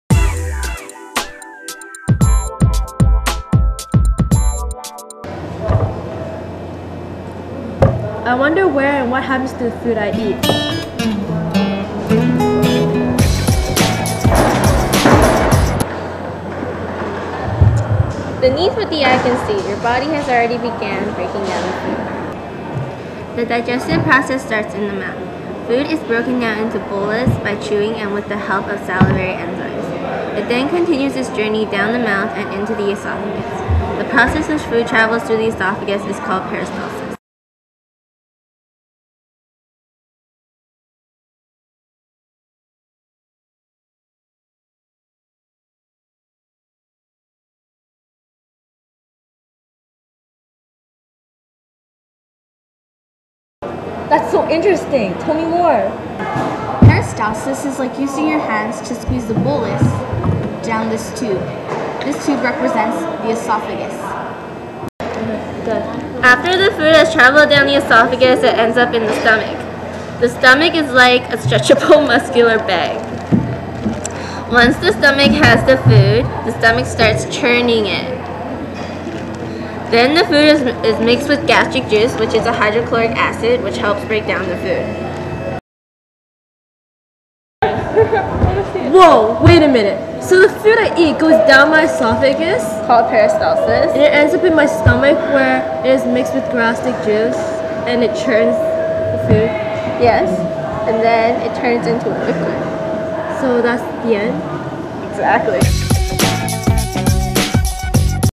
Stage of Digestion
Our topic is the stage of digestion in the whole process of digesting. It starts after food enters the mouth and is broken down into bolus by chewing and salivary enzymes. The bolus then travels down the esophagus in a process called peristalsis. The esophagus is long and muscular. The bolus then enters the stomach which can hold up to 2 litres of food and liquid. Inside the stomach is gastric juice which can help break down the food even more. The bolus is then broken down into chyme. This process can take from two to six hours. Inside the stomach, there are two sphincters which are round muscles that can open to allow material to pass through. If you vomit, the flow of material is reversed. Once the chyme leaves the stomach, it enters the small intestines. Once the chyme leaves the first metre of the small intestine called the duodenum, the stage of digestion is complete.